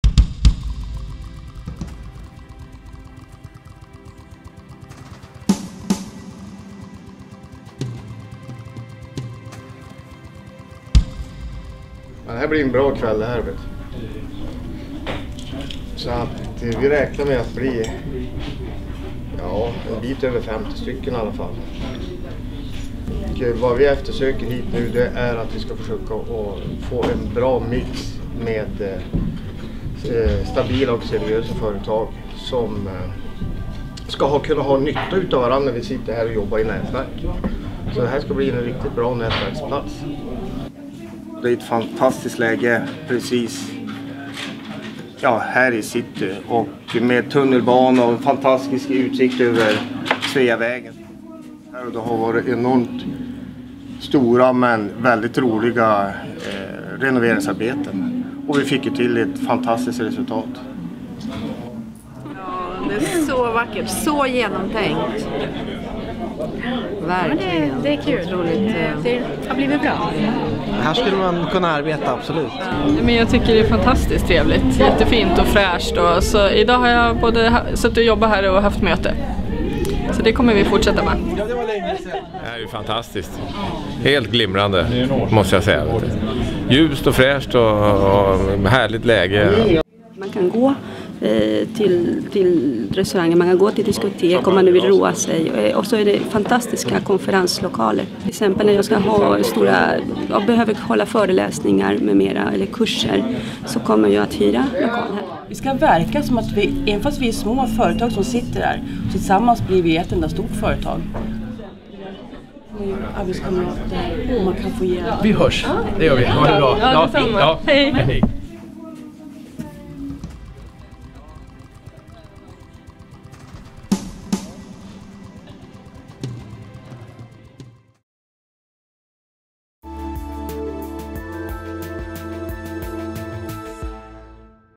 Invigning av KopparCronan Business Lounge, Sveavägen 55 (kortvers)
Invigning av KopparCronan Business Lounge på Sveavägen 55 i Stockholm City. Uthyrning av kontorsarbetsplatser.
Filmen producerad av QL Filmdevelopment